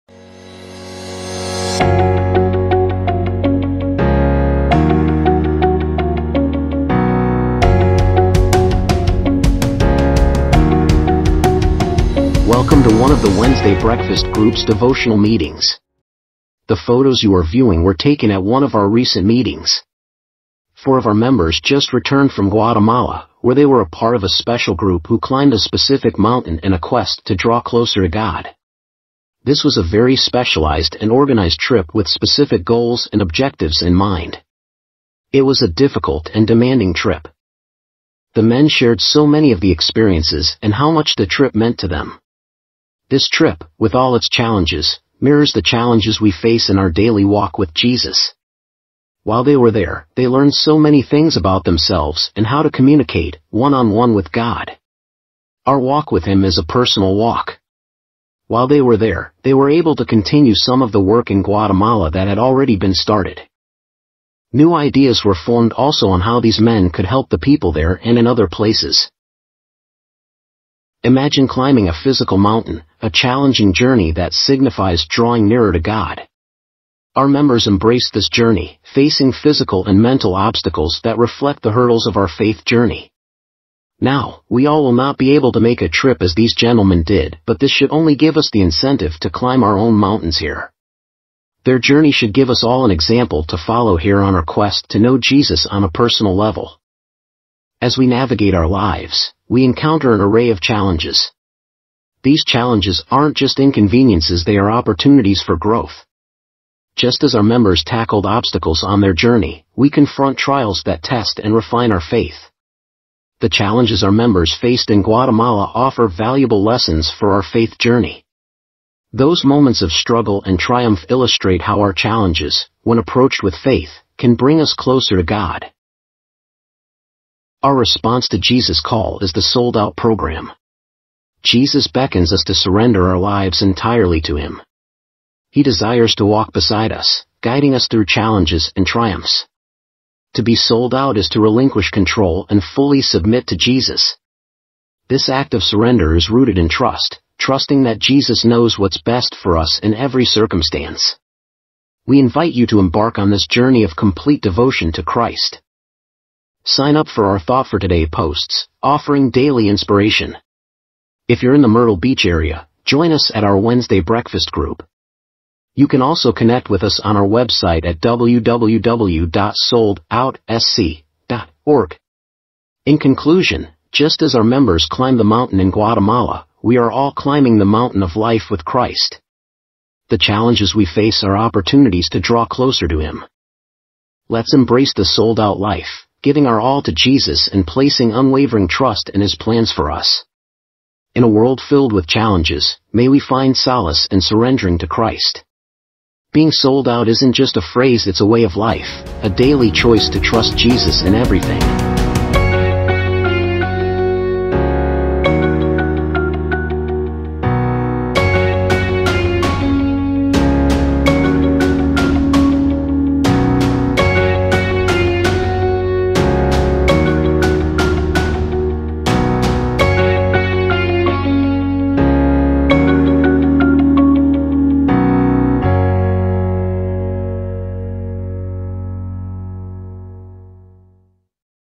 Souled Out Climbing the Mountain
Being "Souled Out" to Jesus, well, it is a way of life and not just a slogan. Watch and hopefully get a better understanding of what this means.